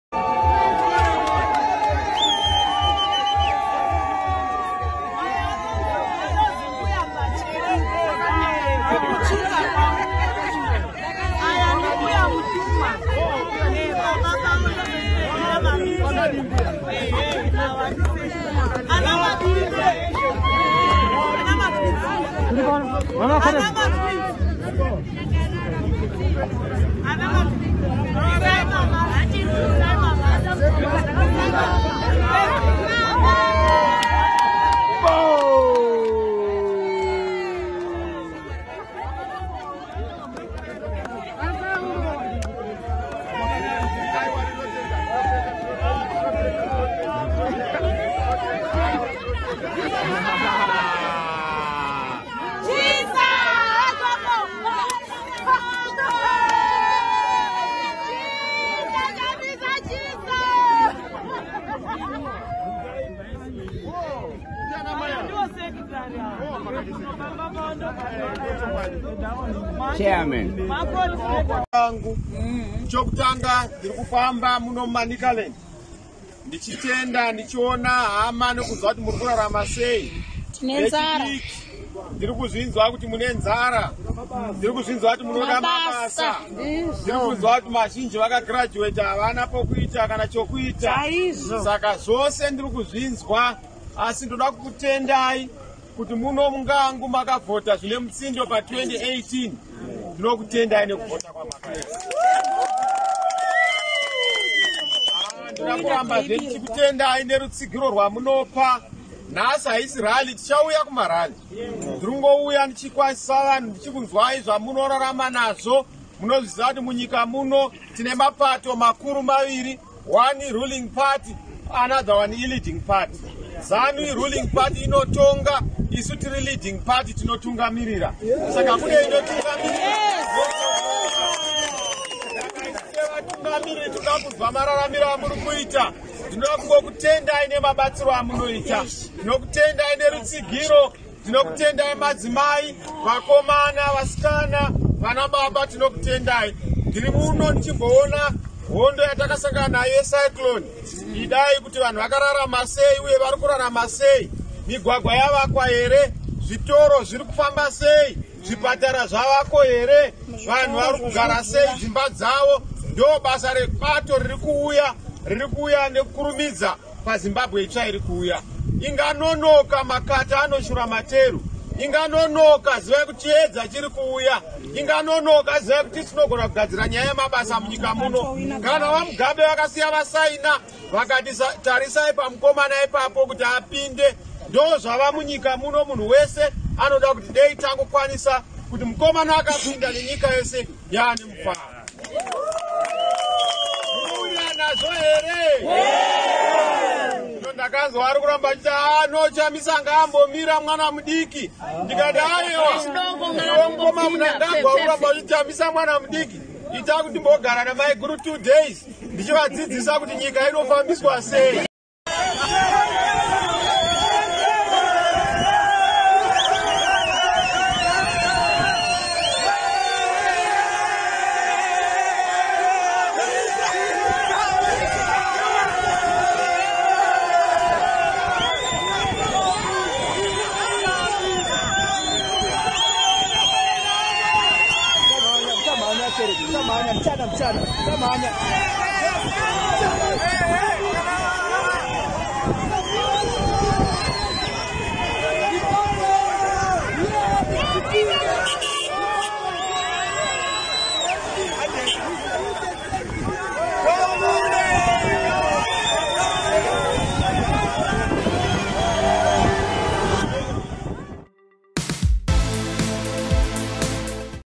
Nelson Chamisa in Manicaland
Nelson Chamisa meeting his party supporters in Manicaland this week.